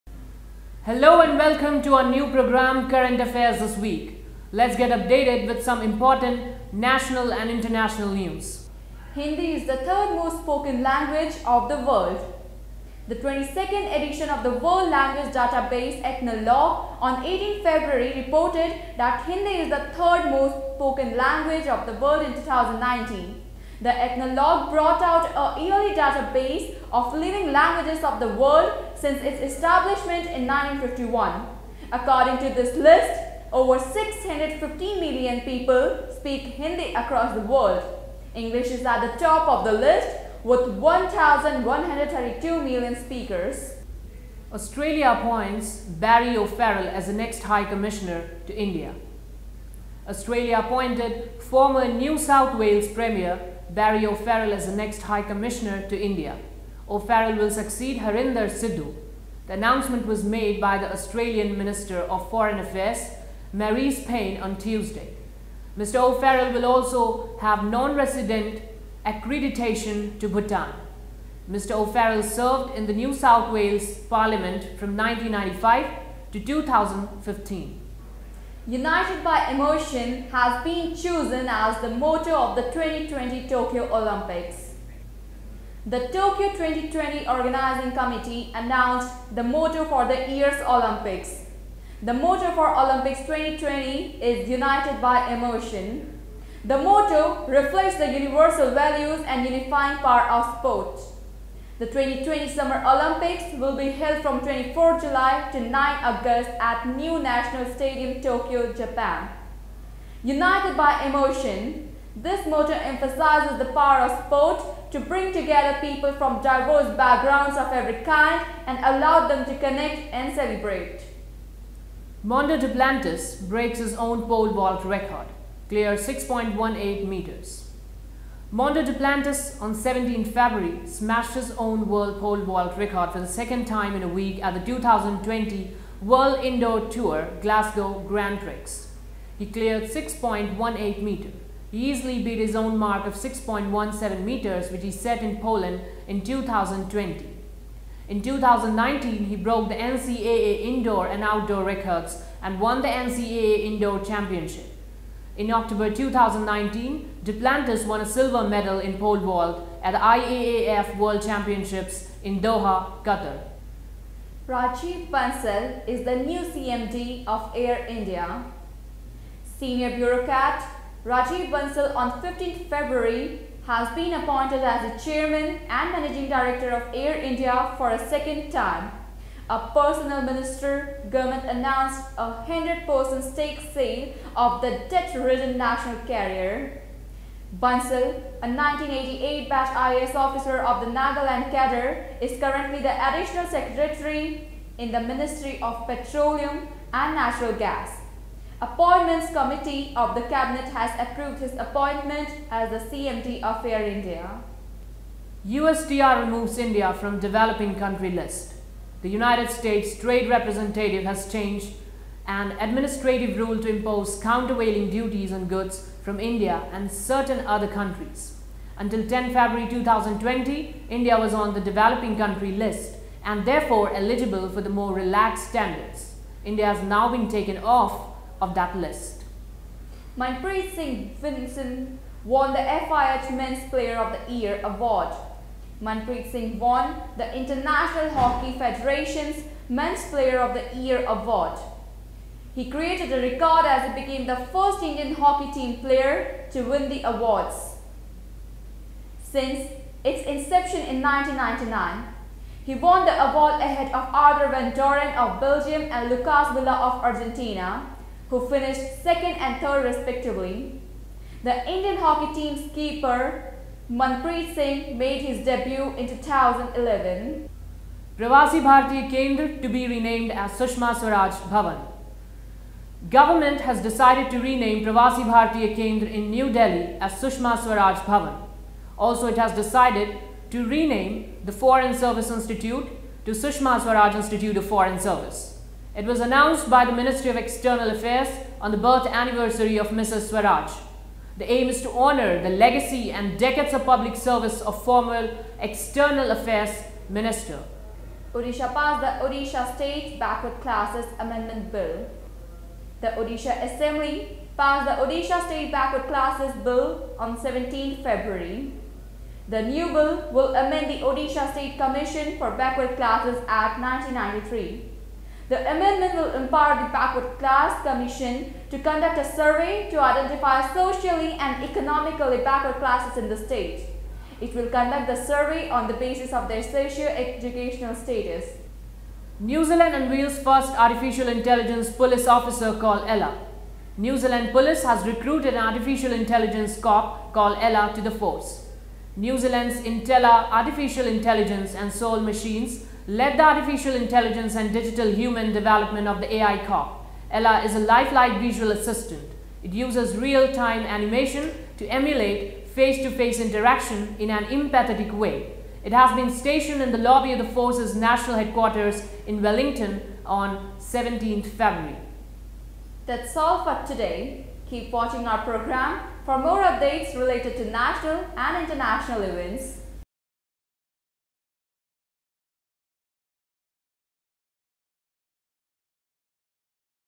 Current Affairs Programme Season 1 Episode 1
This is a weekly current affairs programme, scripted and edited by the students of Jagran Institute of Management and Mass Communication, Noida. The main objective of this program is to update the people with national and international issues.
This programme also aims to give initial and crucial training to the first year students related to the production of news program.
Anchoring: Namra (BJMC 2) & Bhagyaluxmi Yadav (BJMC 2)
Editing & Graphics: Alok Singh (BJMC 6)
Graphics: Arnav Kaushik (BJMC 6)

Topics covered in this episodes are as follow:
1. Hindi is the third most spoken language of the world
2. Australia appoints Barry O’Farrell as the next high commissioner to India
3. “United by Emotion” has been chosen as the motto of the 2020 Tokyo Olympics
4. Mondo Duplantis breaks own pole vault record, clears 6.18 meters.
5. Rajiv Bansal is the new CMD of India
6. USTR removes India from developing country list
7. Manpreet Singh won the FIH mens player of the year award
8. Pravasi Bharatiya Kendra to be renamed as Sushma Swaraj Bhawan
9. Odisha passed the odisha state backward classes amendment bill
10. New Zealand unveils first AI police officer called Ella